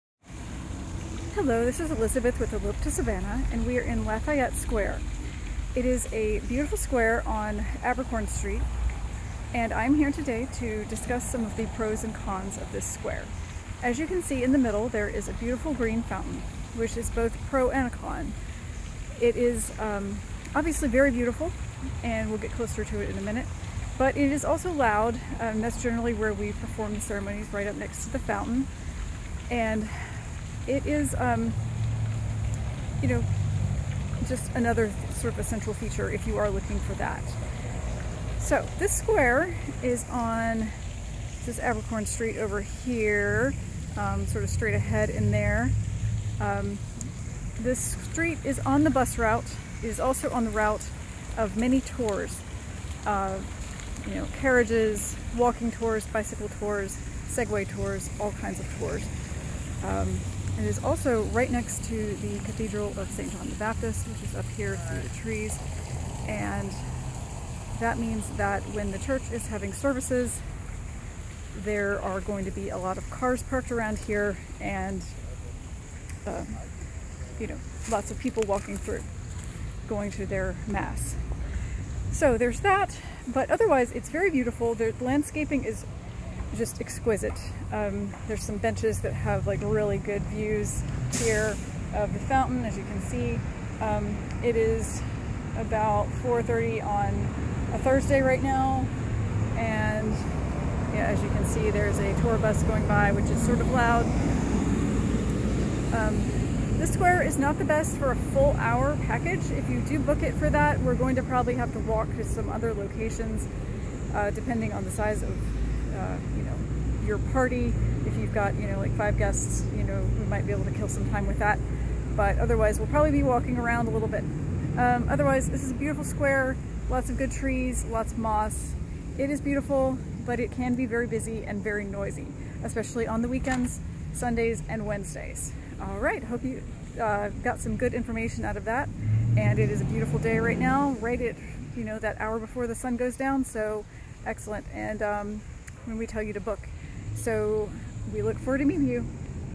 Elope To Savannah - Lafayette square location video updated 2022
Beautiful location on Abercorn Street. We show this square in real life at the Golden hour. List pros and cons.